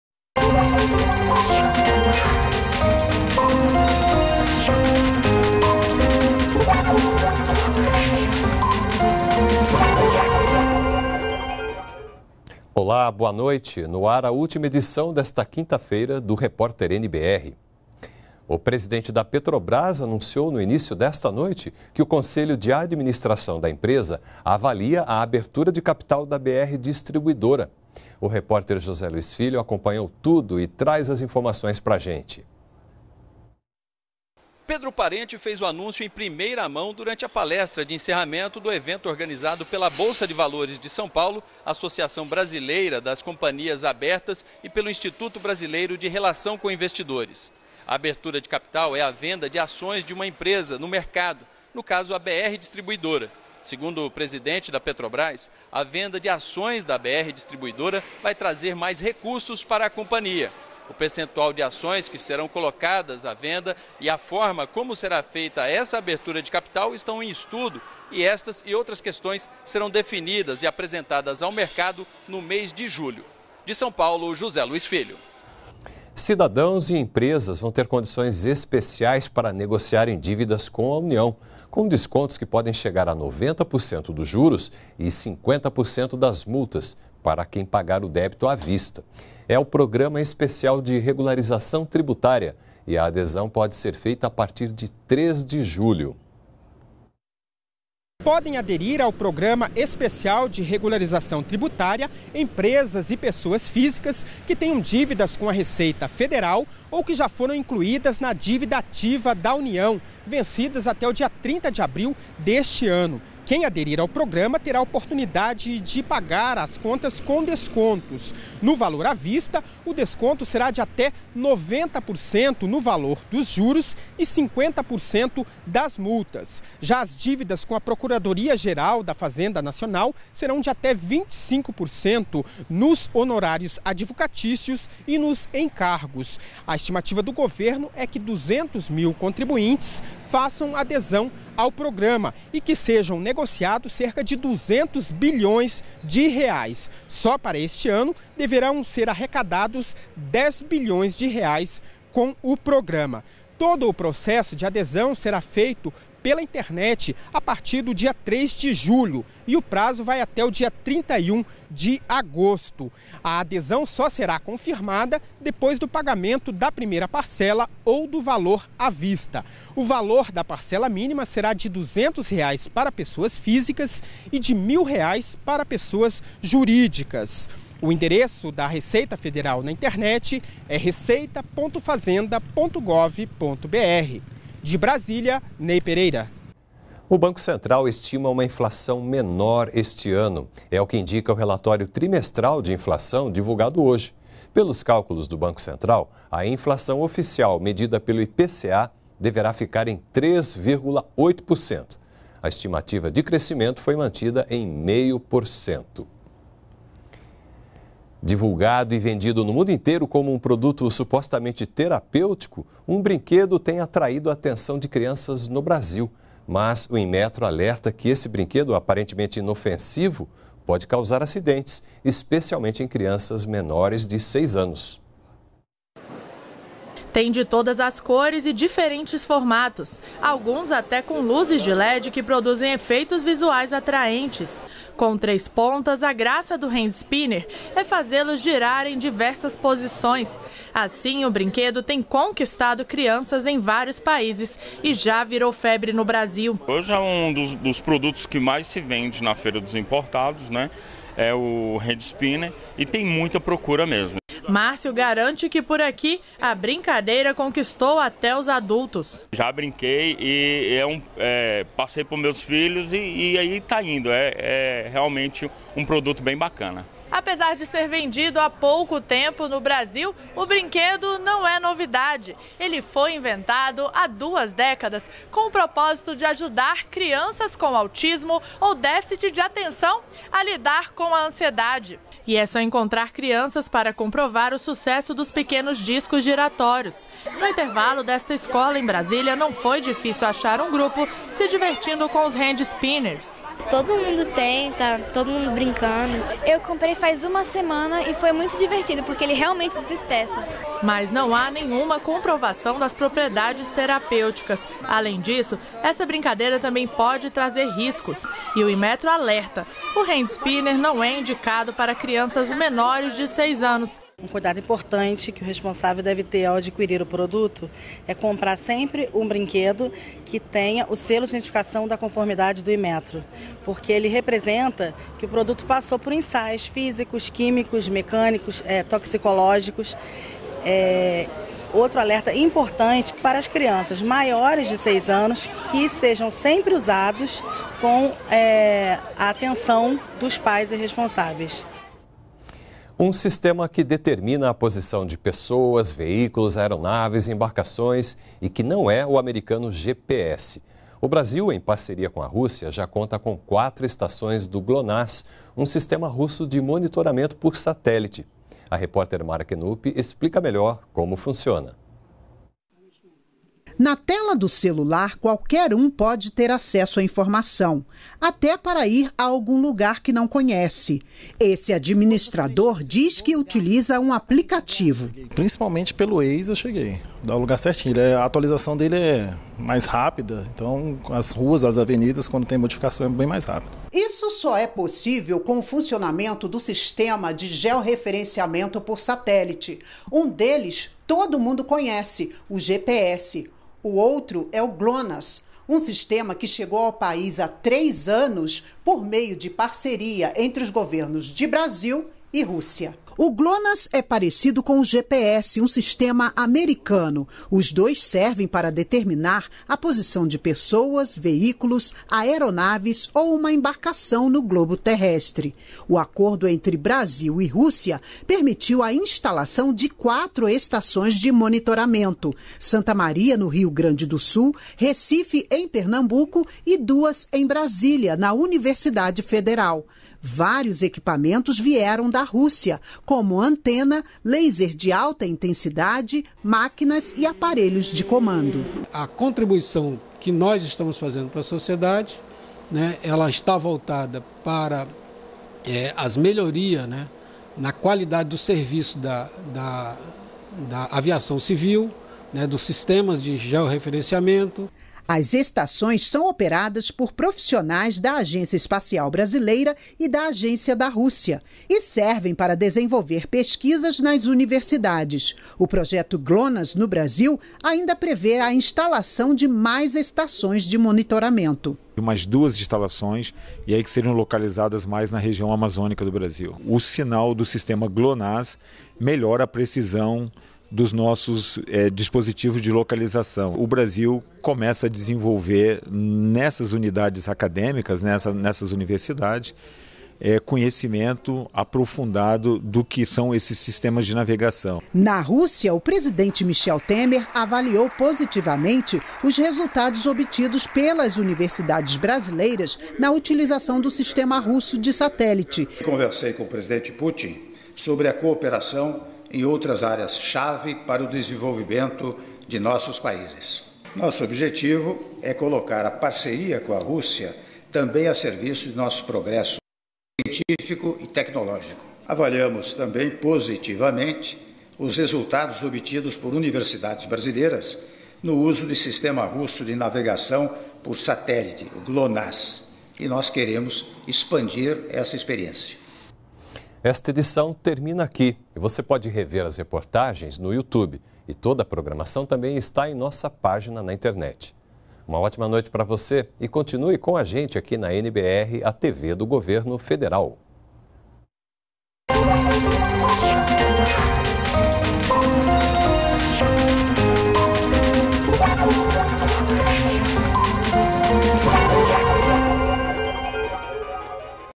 Petrobras avalia abertura de capital da BR Distribuidora
REPÓRTER NBR 20H - 22.06.17: Petrobras anunciou que o conselho de administração da empresa avalia abertura de capital da BR Distribuidora. E mais: empresas e cidadãos podem aderir ao  Programa Especial de Regularização Tributária (Refis) a partir de 3 de julho. Confira também: Inmetro alerta consumidores para a compra e manuseio do programa Hand Spinner.